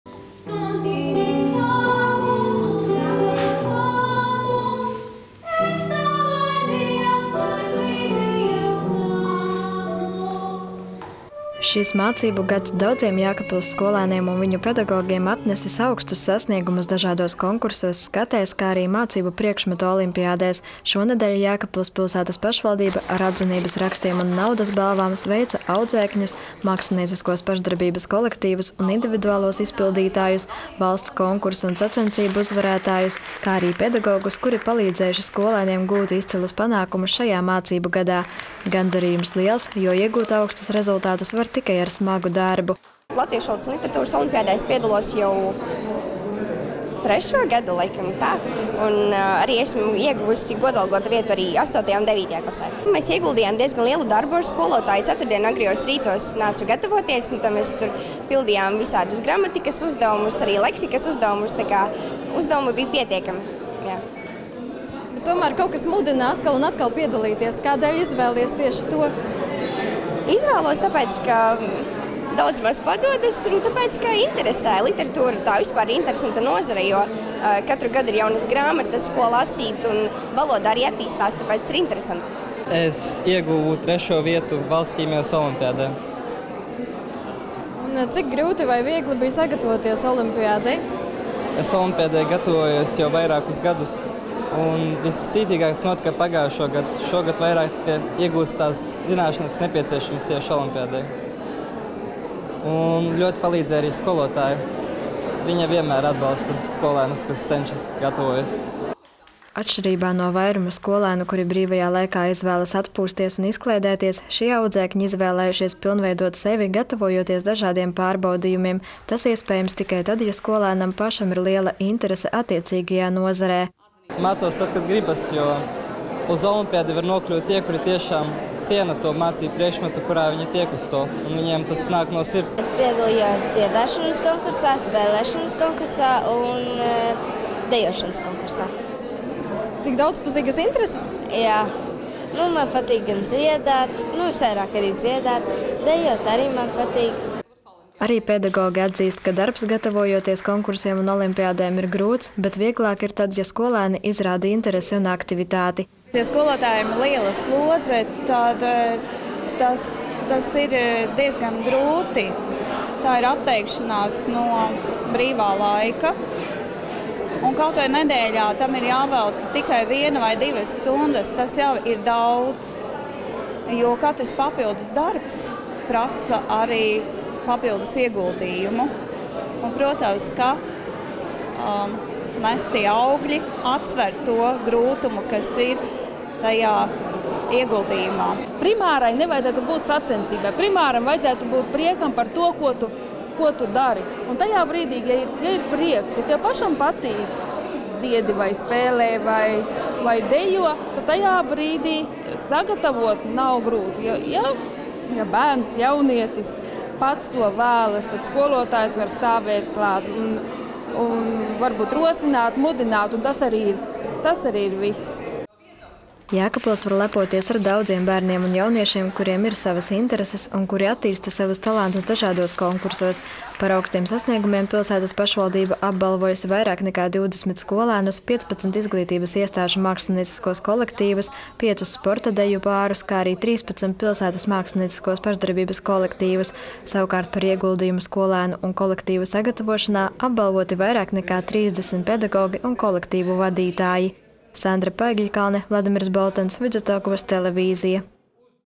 Jēkabpils skolēnus, pedagogus un mākslinieciskos kolektīvus sveic par sasniegumiem
Šis mācību gads daudziem Jēkabpils skolēniem un viņu pedagogiem atnesis augstus sasniegumus dažādos konkursos, skatēs, kā arī mācību priekšmetu olimpiādēs. Šonedēļ Jēkabpils pilsētas pašvaldība ar Atzinības rakstiem un naudas balvām sveica audzēkņus, mākslinieciskos pašdarbības kolektīvus un individuālos izpildītājus, valsts konkursu un sacensību uzvarētājus, kā arī pedagogus, kuri palīdzējuši skolēniem gūt izcilus panākumus šajā mācību gadā.
    Gandarījums liels, jo iegūt augstus rezultātus var tikai ar smagu darbu.

    Jēkabpils Valsts ģimnāzijas 12.klases skolniece Elīna Aļeiņikova, kura ieguvusi 1.vietu valsts latviešu valodas un literatūras olimpiādē, stāsta: „Latviešu valodas un literatūras olimpiādē es piedalos jau trešo gadu. Esmu ieguvusi godalgotu vietu arī astotajā un devītajā klasē. Mēs ar skolotāju ieguldījām diezgan lielu darbu. Es ceturtdienu rītos gāju gatavoties, mēs pildījām visādus gramatikas uzdevumus, arī leksikas uzdevumus. Uzdevumu bija pietiekami daudz. Piedalīties izvēlos tāpēc, ka šis priekšmets daudzmaz padodas un interesē. Literatūra vispār ir interesanta nozare, jo katru gadu ir jaunas grāmatas, ko lasīt, arī valoda attīstās. Tāpēc ir interesanti."

    Jēkabpils Valsts ģimnāzijas skolnieks Edijs Kauranens par saviem sasniegumiem stāsta: „Es ieguvu trešo vietu valsts ķīmijas olimpiādē. Olimpiādei gatavojos jau vairākus gadus. Viscītīgāk tas notika pagājušogad. Šogad vēl vairāk ieguvu zināšanas, kas nepieciešamas tieši olimpiādei. Ļoti palīdzēja skolotāja. Viņa vienmēr atbalsta skolēnus, kas cenšas un gatavojas."

    Atšķirībā no vairuma skolēnu, kuri brīvajā laikā izvēlas atpūsties un izklaidēties, šie audzēkņi izvēlējušies pilnveidot sevi, gatavojoties dažādiem pārbaudījumiem. Tas iespējams tikai tad, ja skolēnam pašam ir liela interese attiecīgajā nozarē.

    Edijs Kauranens skaidro: „Mācos tad, kad gribas, jo uz olimpiādi var nokļūt tie, kuri tiešām ciena to mācību priekšmetu. Un viņiem tas nāk no sirds."

    Jēkabpils Bērnu un jauniešu centra folkloras kopas „Raksti" dalībniece Dārta Vilmane šajā mācību gadā piedalījusies vairākos konkursos un ieguvusi arī godalgotas vietas. Viņa stāsta: „Es piedalījos dziedāšanas konkursā, spēlēšanas konkursā un dejošanas konkursā. Visvairāk man patīk dziedāt, bet dejot arī man patīk."

    Arī pedagogi atzīst, ka darbs, gatavojoties konkursiem un olimpiādēm ir grūts, bet vieglāk ir tad, ja skolēni izrāda interesi un aktivitāti.

    Jēkabpils Valsts ģimnāzijas latviešu valodas un literatūras skolotāja Kristīne Modnika skaidro: „Ja skolotājam ir liela slodze, tas ir diezgan grūti. Tā ir atteikšanās no brīvā laika. Ja nedēļā tam jāvelta tikai viena vai divas stundas, tas jau ir daudz. Katrs papildu darbs prasa arī papildu ieguldījumu. Bet, protams, ka nestie augļi atsver to grūtumu, kas ir tajā ieguldījumā."

    Jēkabpils Bērnu un jauniešu centra folkloras kopas „Raksti" vadītāja Vita Talla pauž savu viedokli: „Primārai nevajadzētu būt sacensībai, primāram vajadzētu būt priekam par to, ko tu dari. Ja ir prieks, ja pašam patīk -- vienalga, vai tu dziedi, spēlē vai dejo, tajā brīdī sagatavot skolēnu konkursam nav grūti. Ja bērns pats to vēlas, tad skolotājs var stāvēt klāt un tikai rosināt un mudināt. Tas arī viss."

    Jēkabpils var lepoties ar daudziem bērniem un jauniešiem, kuriem ir savas intereses un kuri attīsta savus talantus dažādos konkursos. Par augstiem sasniegumiem pilsētas pašvaldība apbalvojusi vairāk nekā 20 skolēnus, 15 izglītības iestāžu mākslinieciskos kolektīvus, piecus sporta deju pārus, kā arī 13 pilsētas mākslinieciskos pašdarbības kolektīvus. Savukārt par ieguldījumu skolēnu un kolektīvu sagatavošanā apbalvoti vairāk nekā 30 pedagogi un kolektīvu vadītāji. Pielikumā saraksti ar visiem apbalvojumu ieguvējiem, kā arī pašvaldības apbalvošanas noteikumi.

    Sandra Paegļkalne